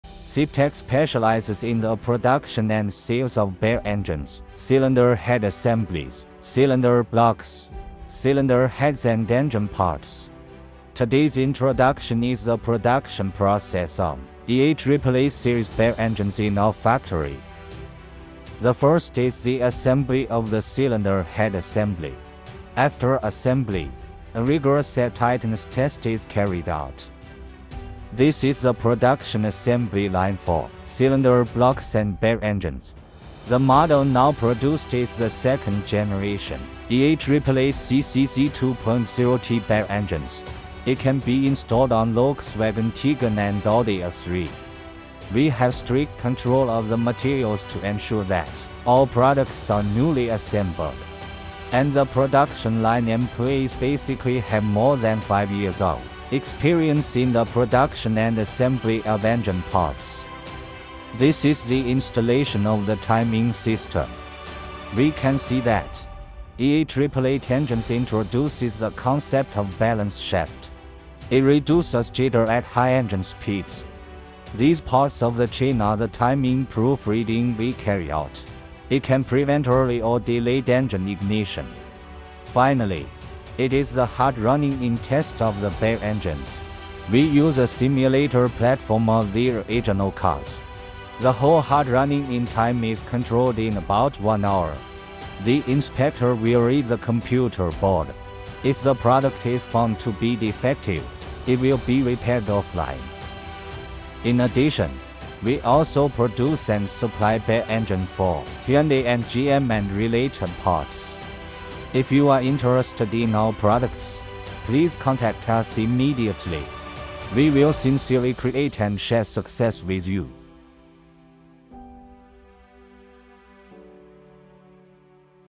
The production process of the EA888 series engine
Gen2 EA888 CCZ 2.0T engine